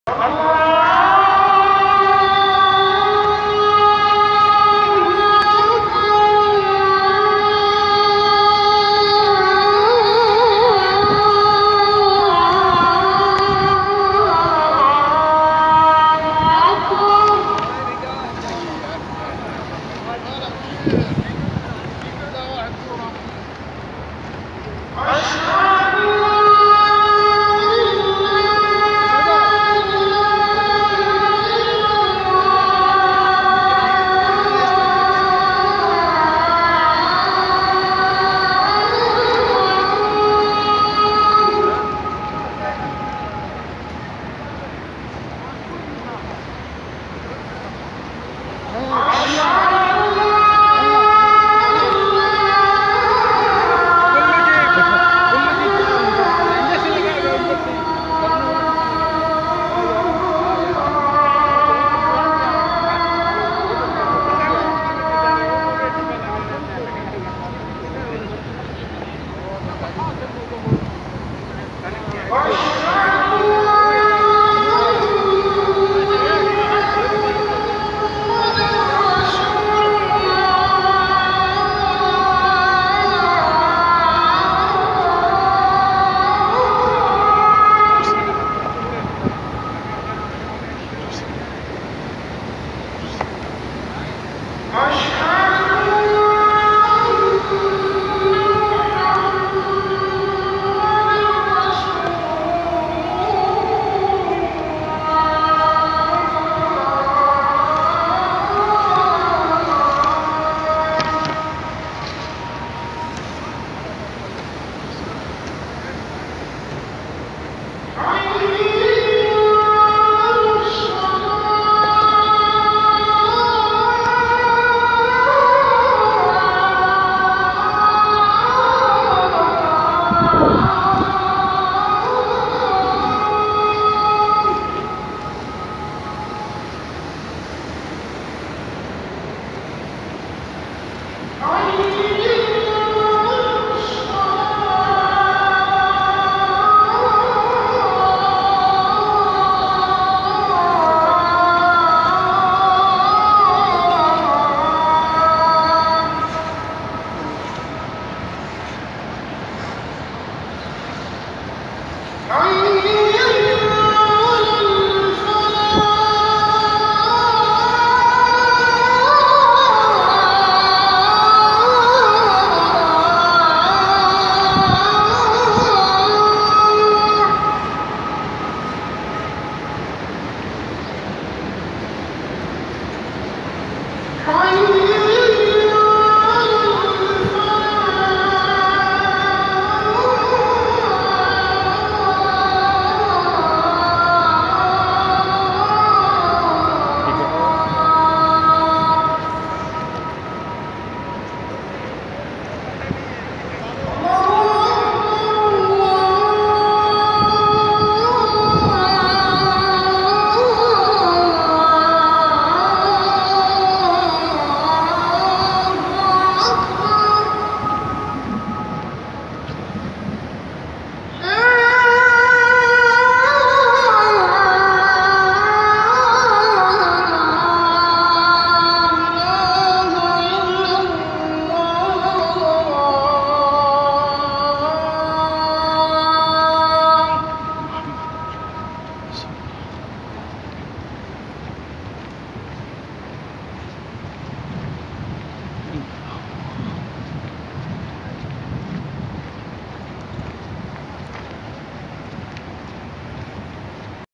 AZAN zohar in MADINA ||آذان ظہر مسجد نبوی شریف
Azaan zohar in MADINA MASJID al NABVI shareef with BEAUTIFUL VIEW of MASJID al NABVI